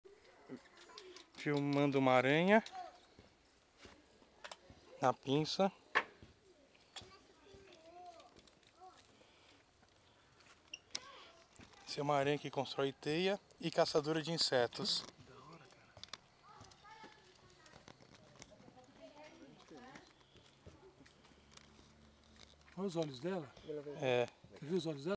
Paulo Aníbal na coleta de aranhas
Um pequeno trecho de coleta de aracnídeos filmado por uma TV Aberta brasileira nas matas da Serra da Cantareira, com o biólogo
Paulo Anibal G. Mesquita.